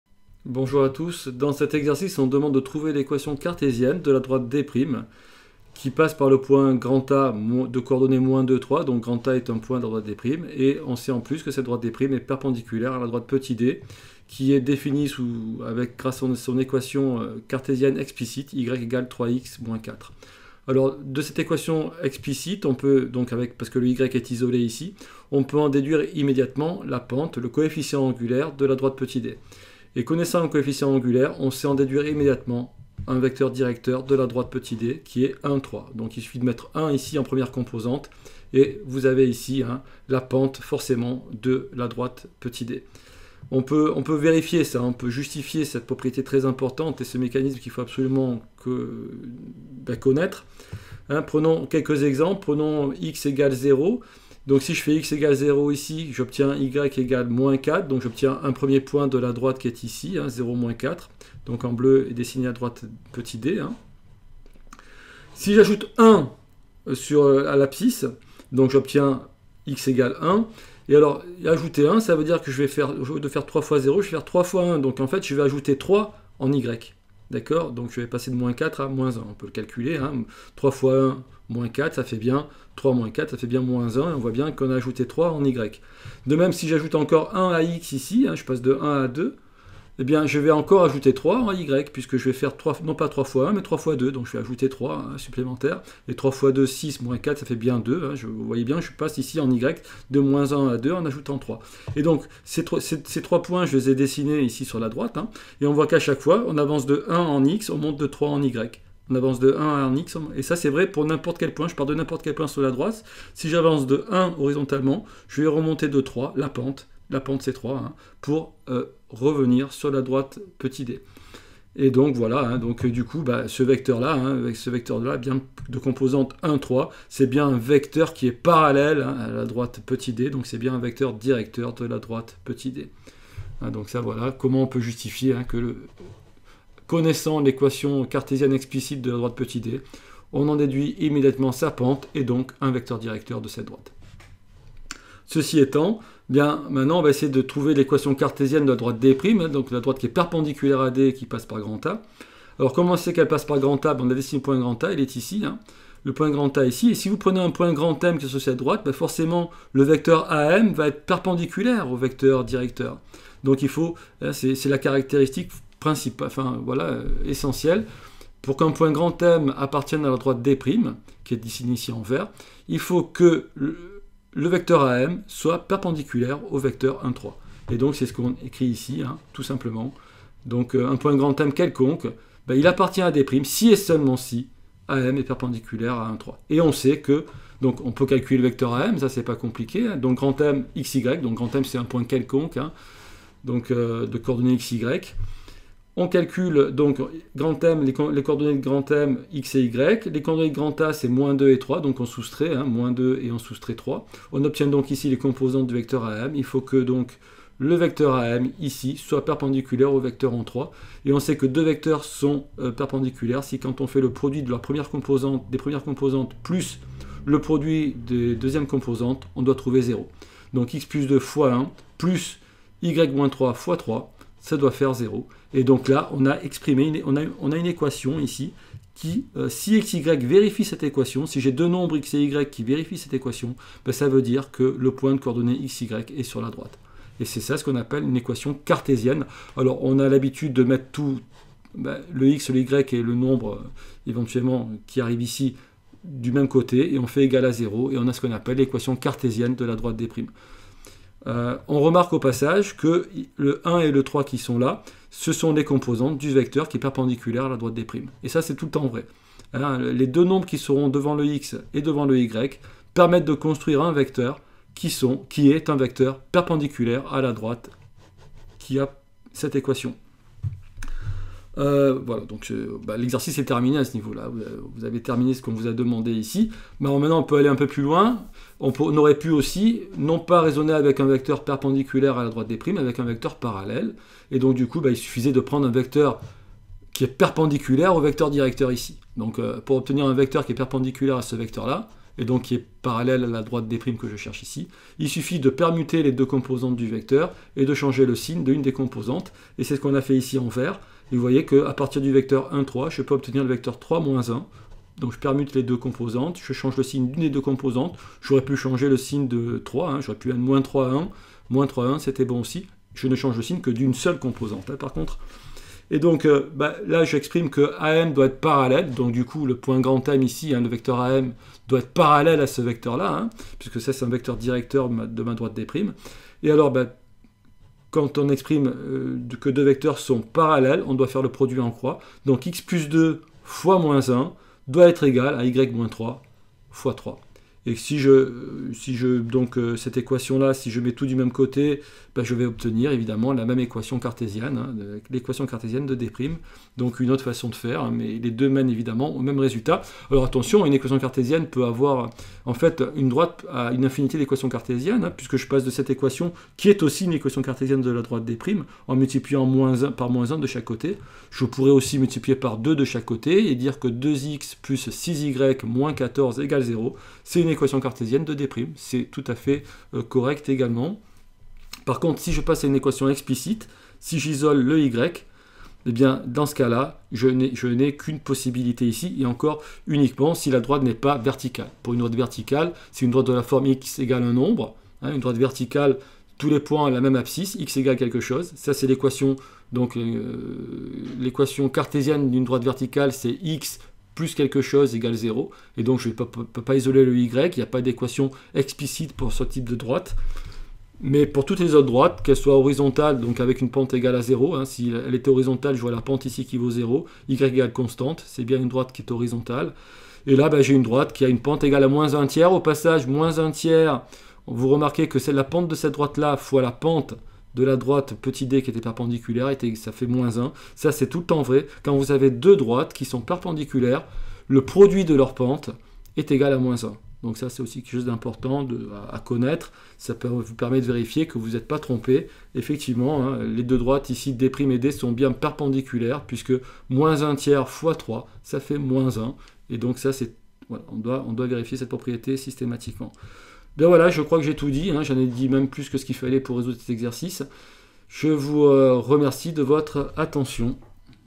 Equation cartésienne d'une droite passant par un point et perpendiculaire à une droite donnés.
Avec rappel et illustration des propriétés du coefficient angulaire d'une droite.